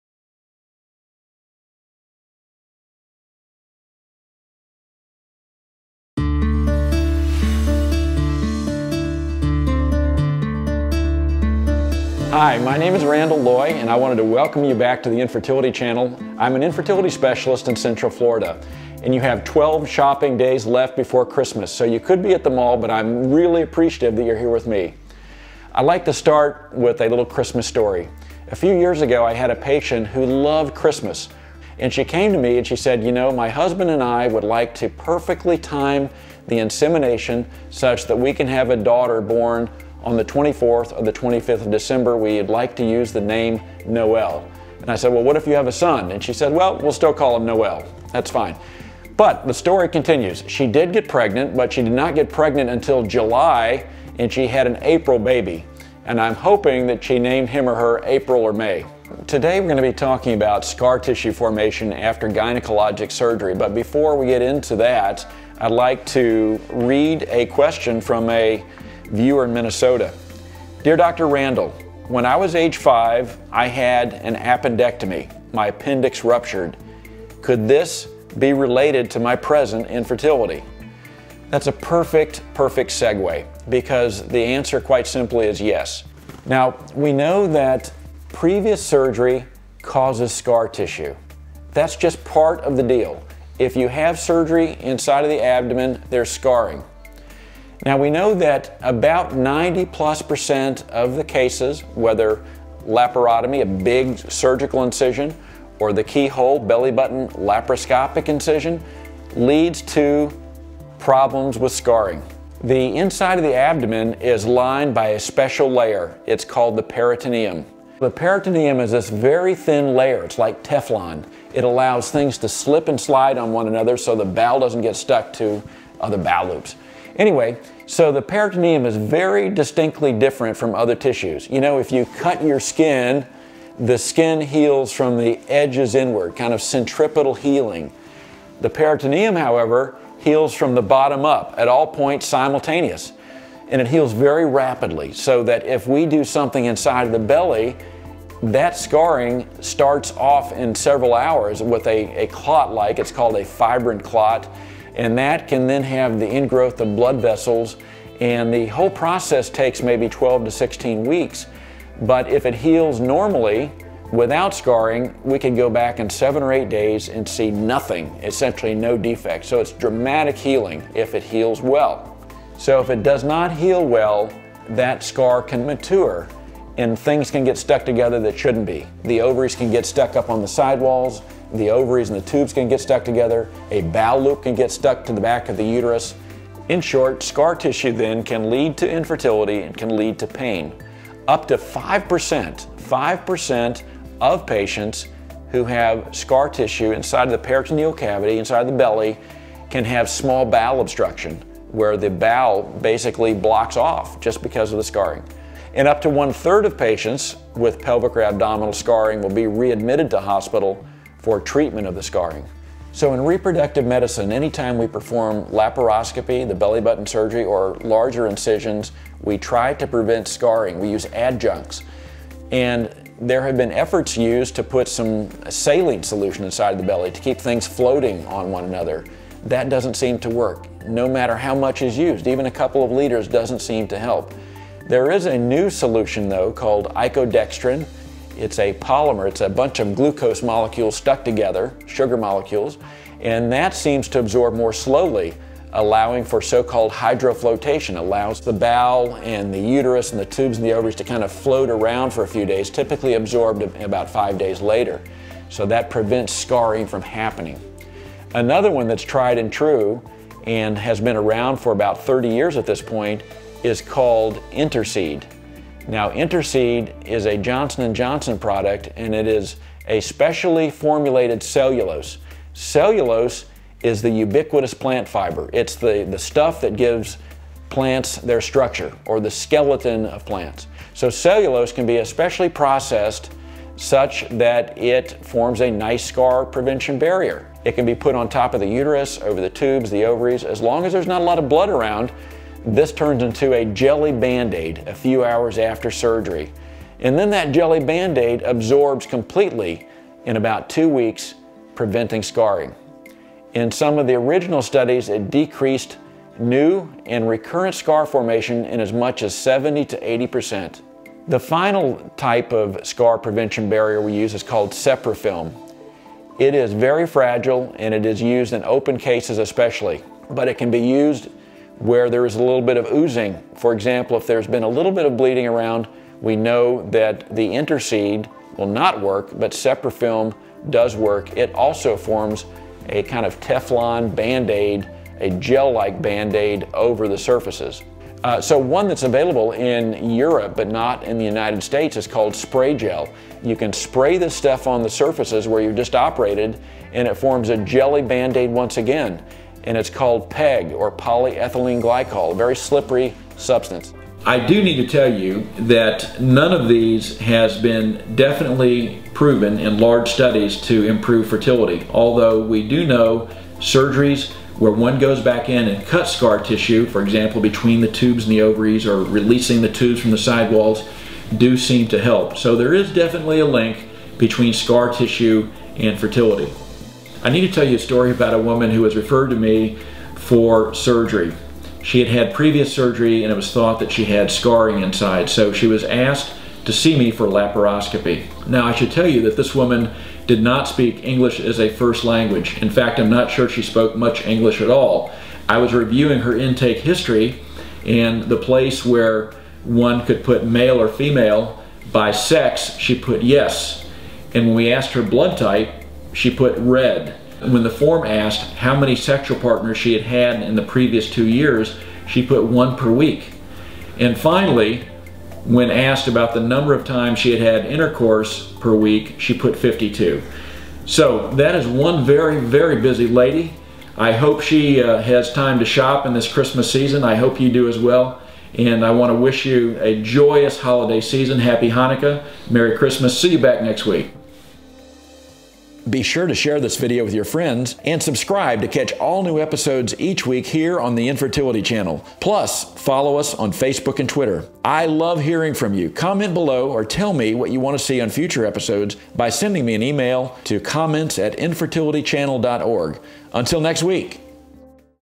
Scar-y, Scar-y Night: Surgery and Scar Prevention
Surgery can be scary! Scar tissue, the adhesions that can form after surgery, can be scarier still. There is a better chance to conceive when pesky scar tissue doesn't interfere.

Dr. Randall A. Loy shares information about special barriers—adjuncts—that can be used during gynecological surgery to help prevent scar tissue from forming.